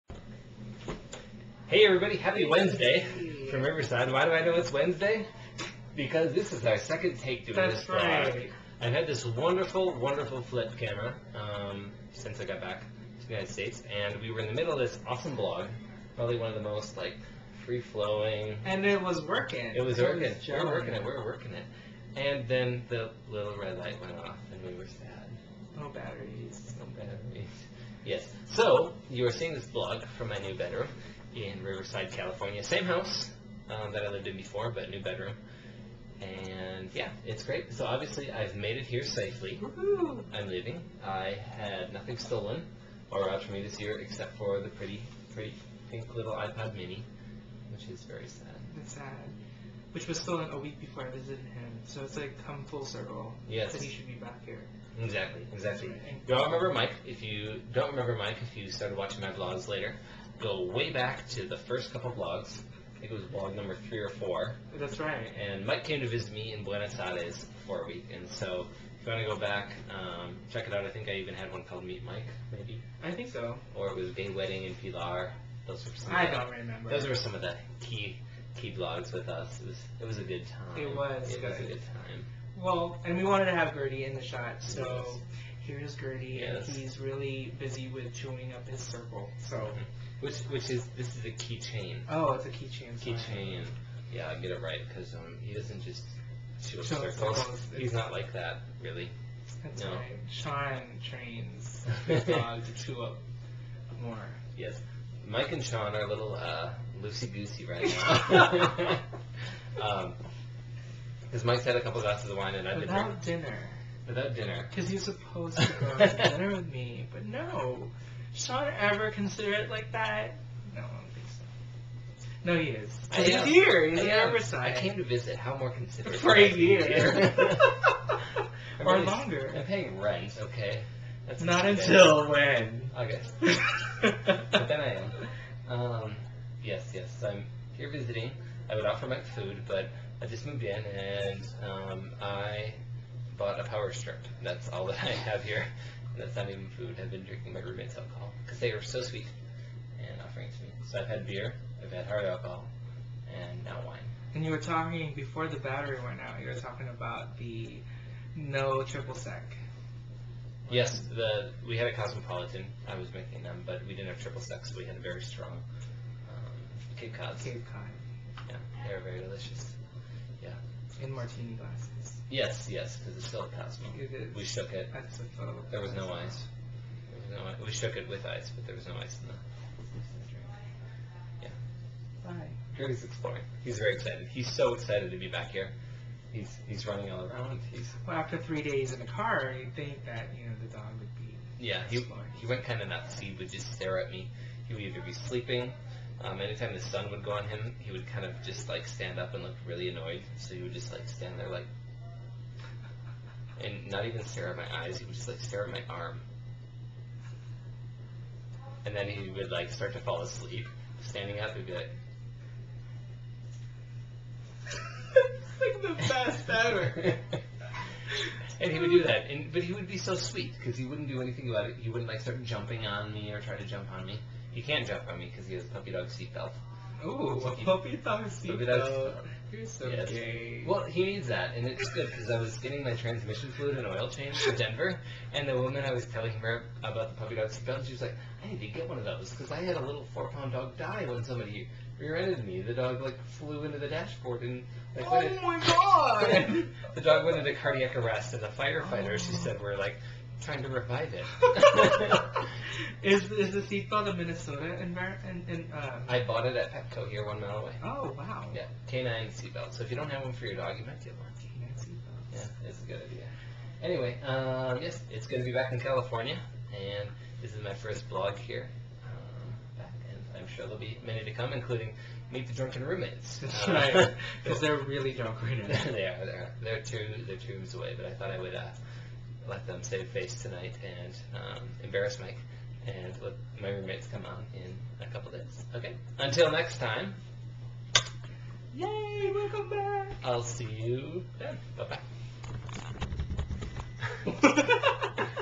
First night back in Riverside!
Chatting with Mike on my first night back in Riverside